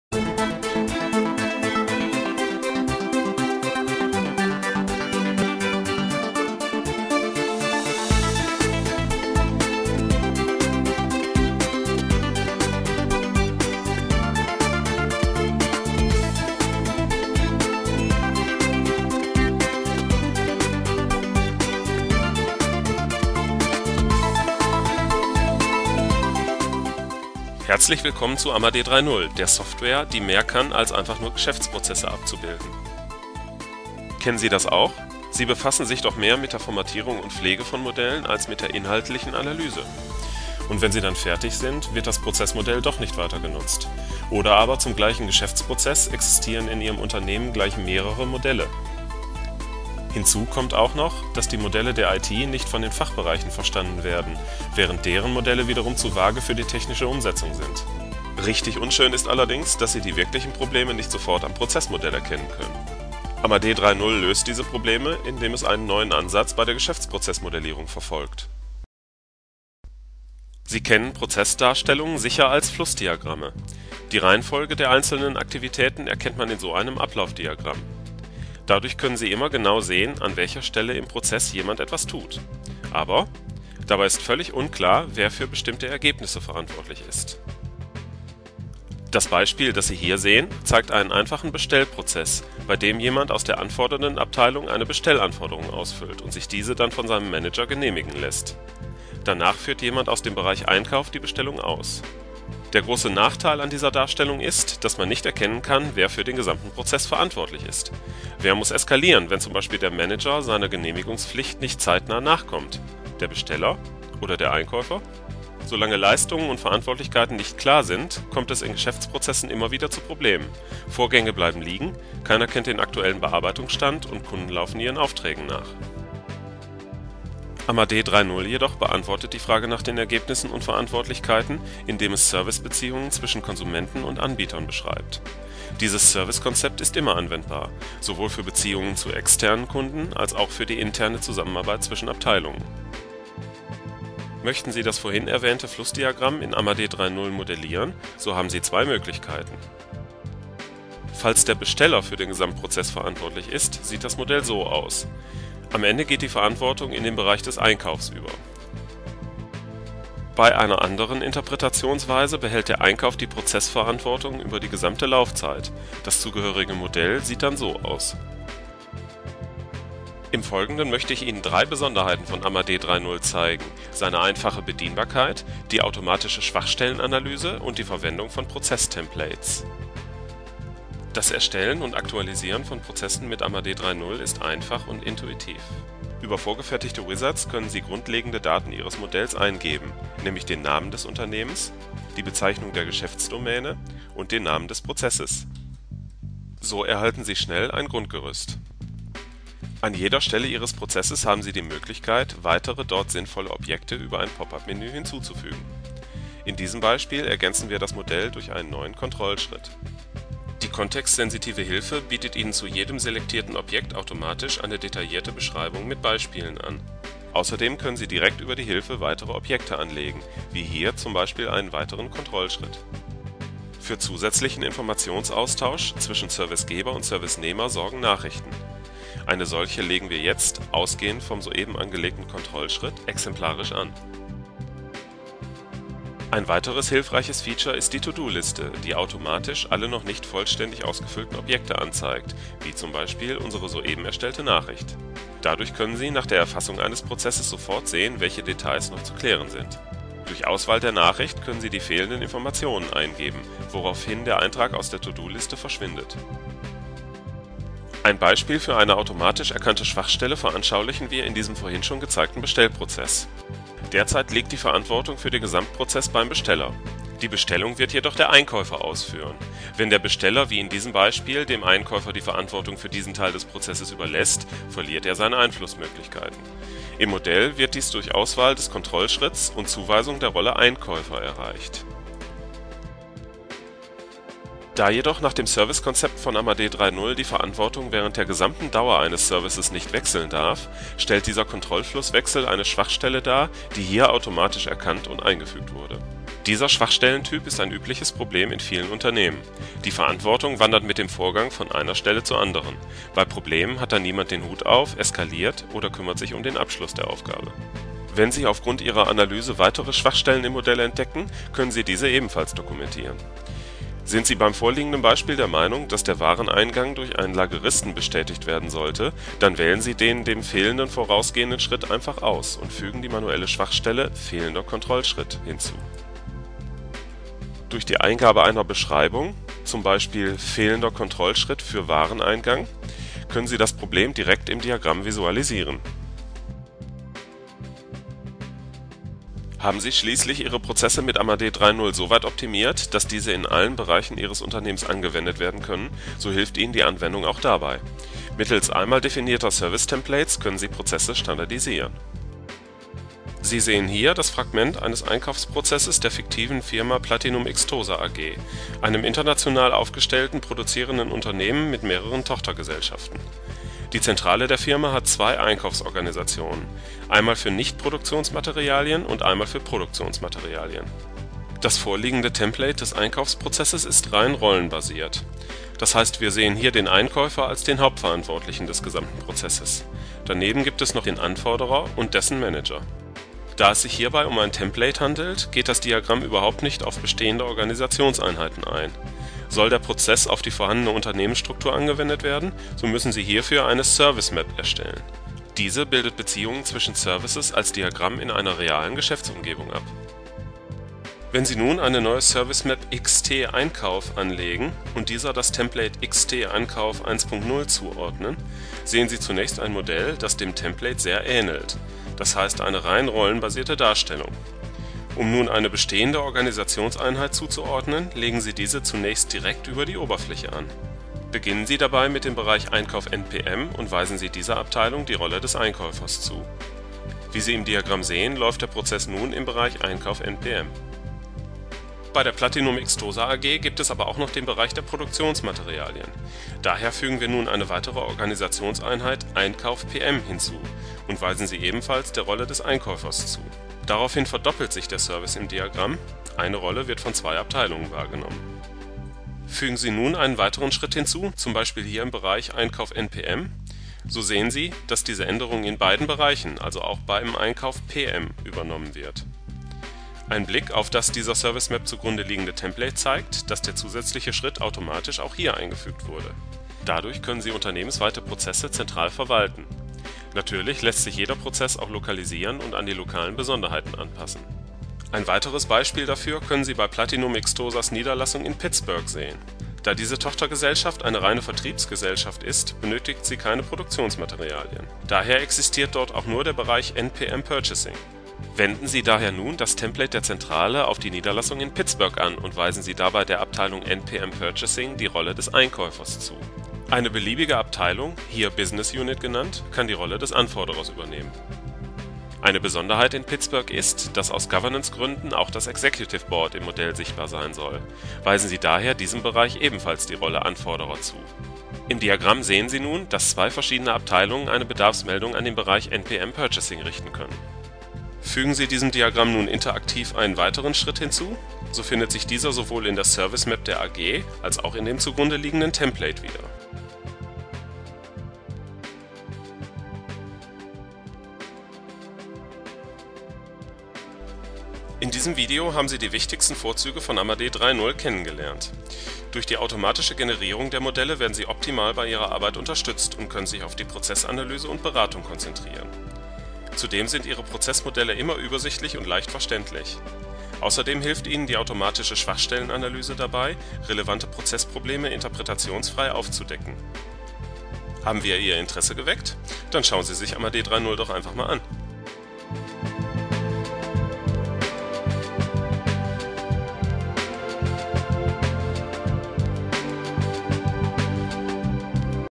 AMADEE 3.0 Produktvorstellung - HD
Dieses Video gibt Ihnen eine kurze Einführung in den AMADEE 3.0 Enterprise Designer